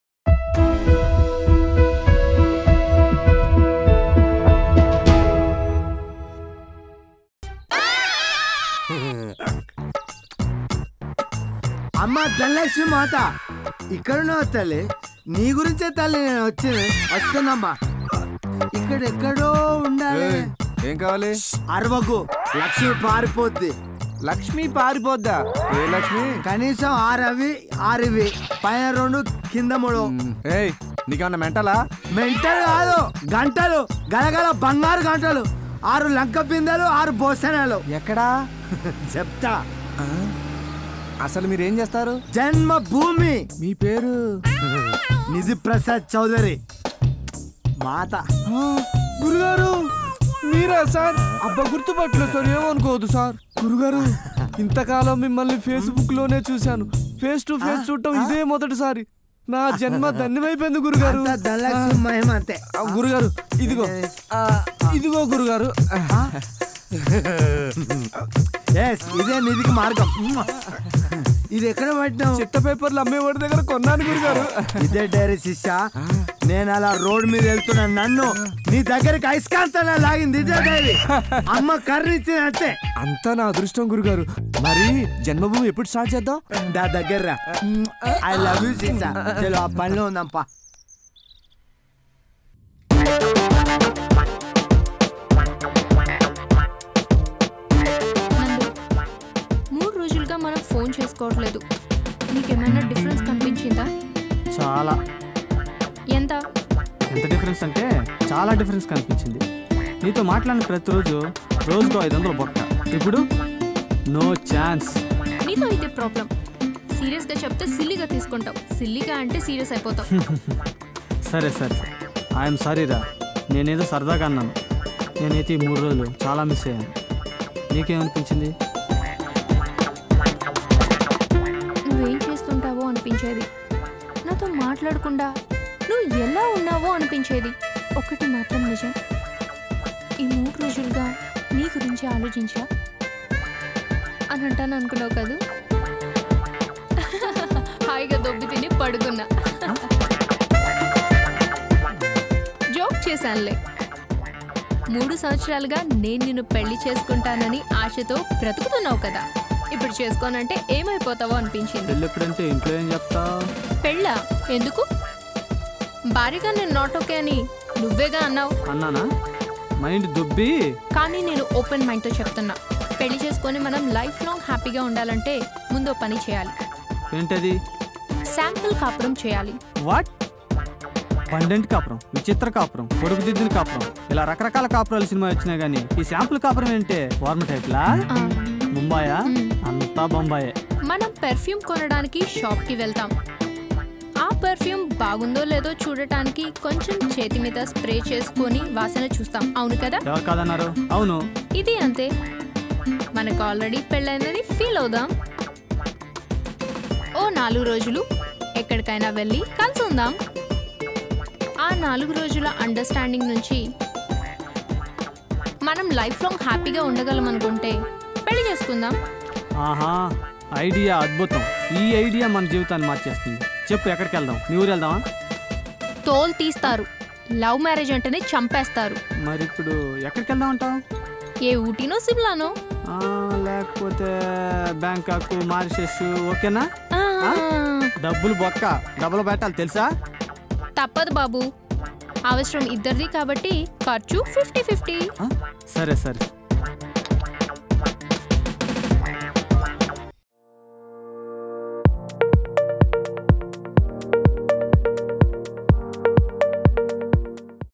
Sivaji & Reethukour Funny Argument Scene | TFC Filmnagar
Watch & Enjoy Sivaji & Reethukour Funny Argument Scene. Keep Follow Us For More Latest Telugu Movie Scenes.

MOVIES

NEWS

WEBSITES